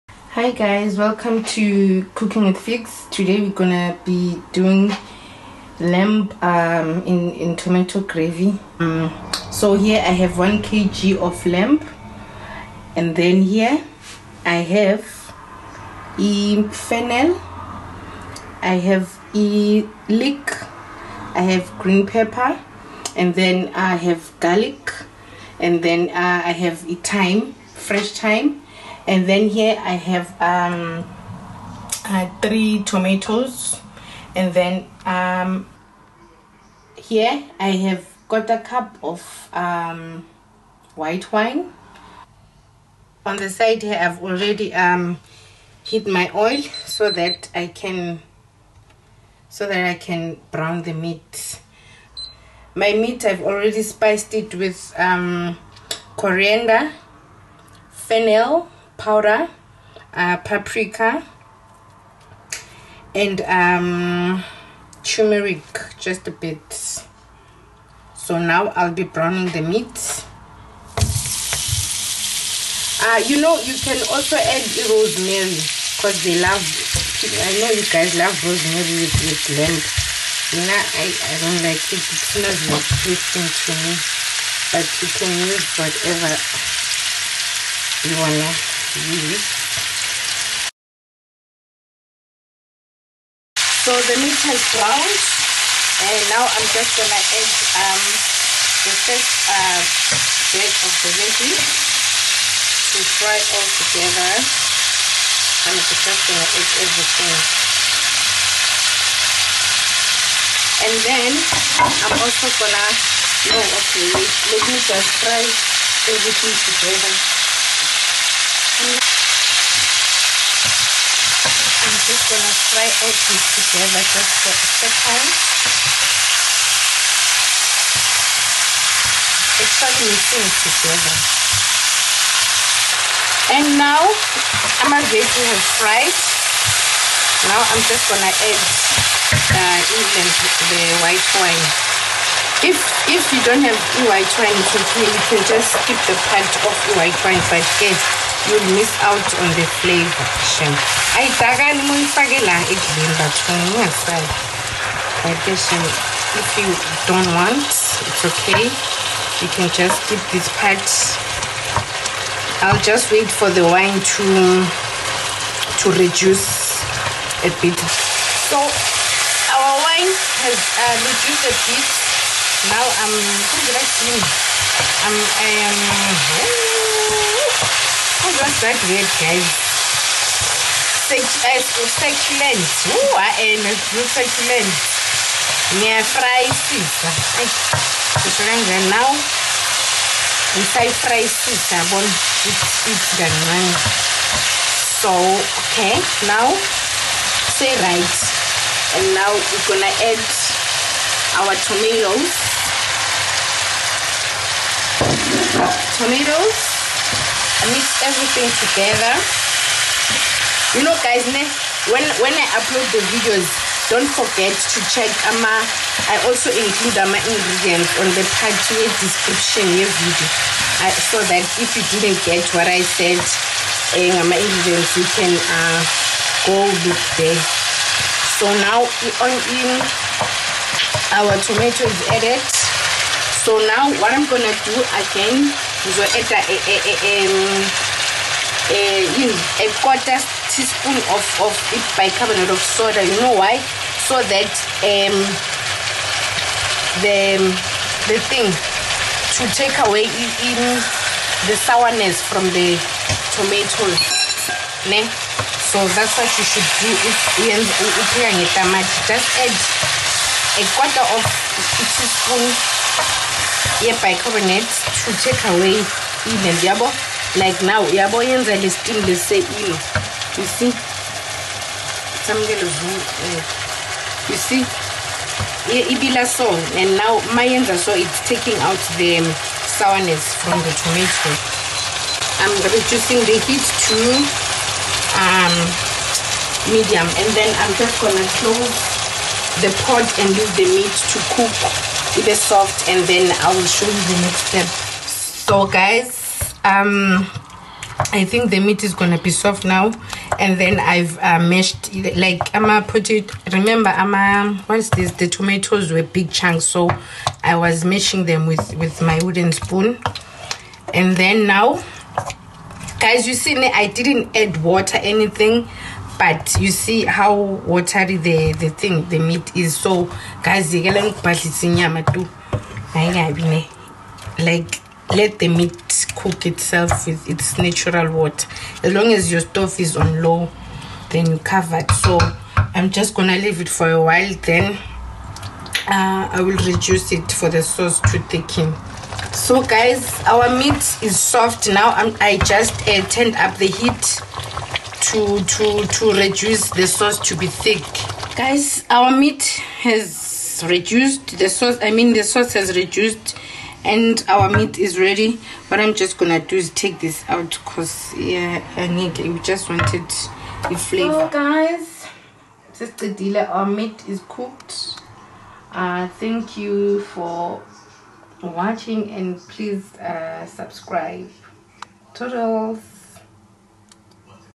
Lamb In Tomato Gravy
1 kg lamb
3 tomatoes roughly chopped
1/2 fennel chopped
2 leeks chopped
3 garlic cloves minced
1/4 green pepper
fresh thyme
1/4 cup of white dry wine
spices :Tumeric ,paprika,corriander and fennel powder